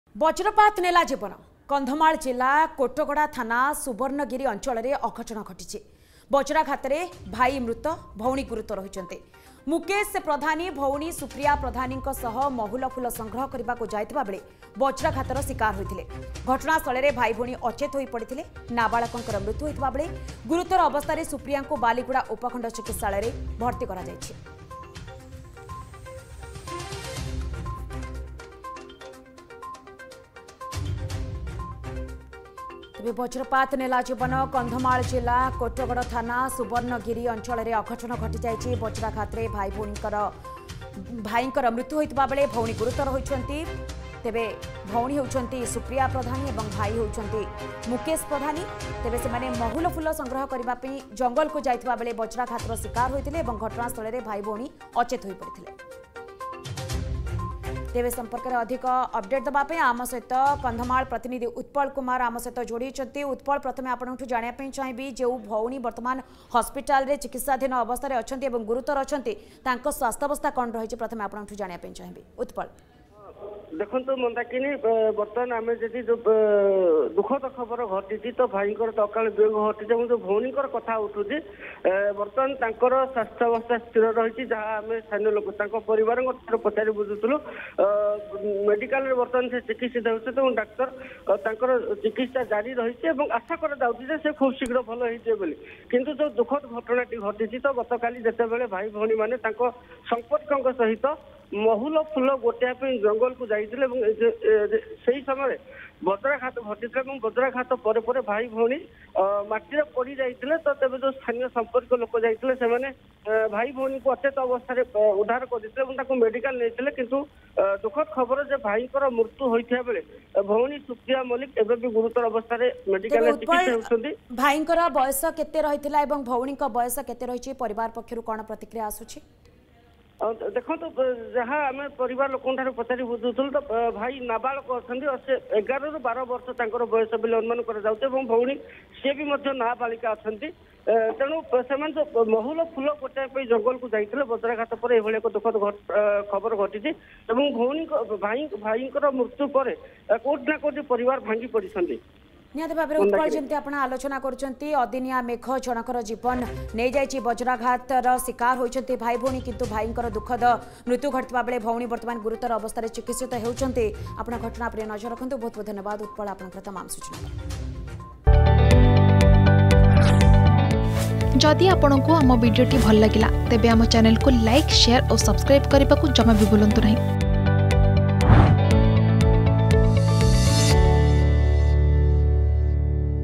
ବଜ୍ରାଘାତରେ ଭାଇ ମୃତ ଭଉଣୀ ଗୁରୁତର || Lightning Strike || Kandhamal
ବଜ୍ରାଘାତରେ ଭାଇ ମୃତ ଭଉଣୀ ଗୁରୁତର । କନ୍ଧମାଳ ଜିଲ୍ଲା କୋଟଗଡ଼ ଥାନା ସୁବର୍ଣ୍ଣଗିରି ଅଞ୍ଚଳରେ ଅଘଟଣ ।